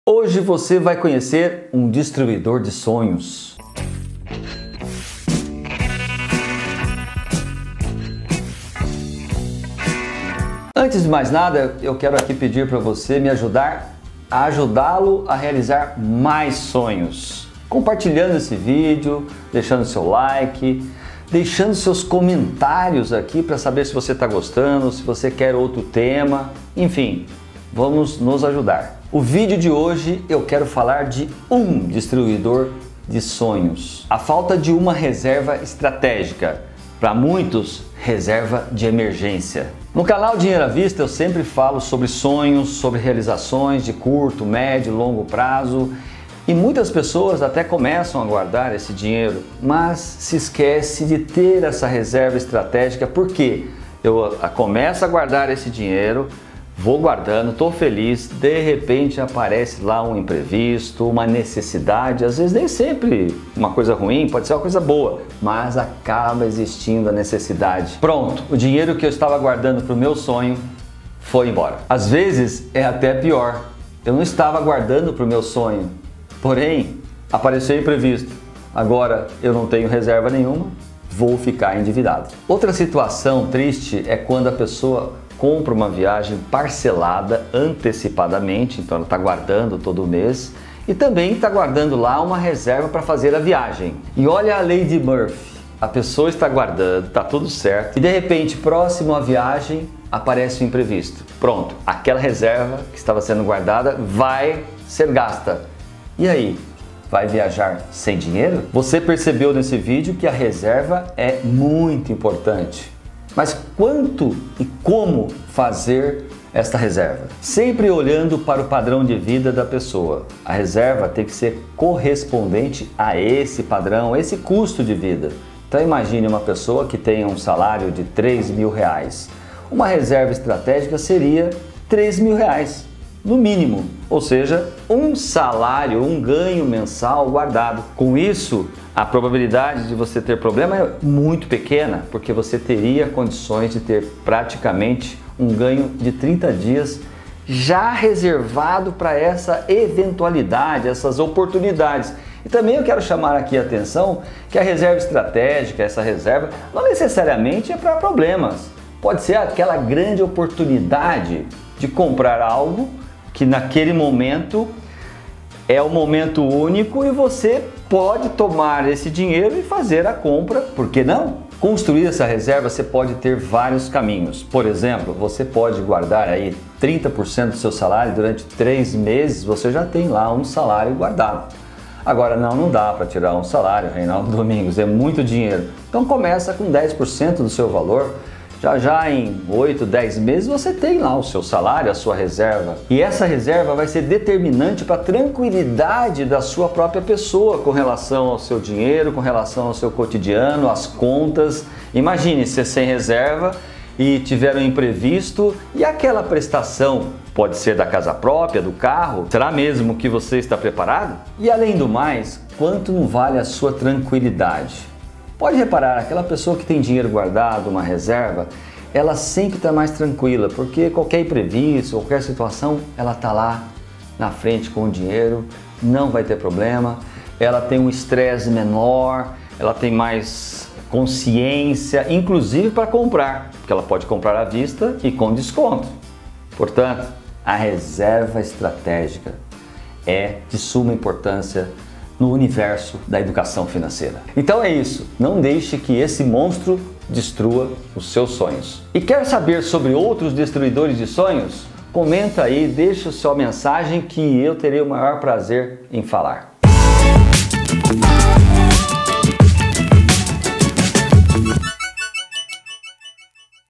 FALTA DE RESERVAS uma grande DESTRUIDORA DE SONHOS
Quer que fale sobre outros destruidores de sonhos, deixe no comentário que farei!
Neste vídeo falo sogre como construir uma reserva estratégica, que hoje é um fator muito importante para ter fôlego para realização de sonhos. Quer saber como ter essa educação financeira? Assista o vídeo!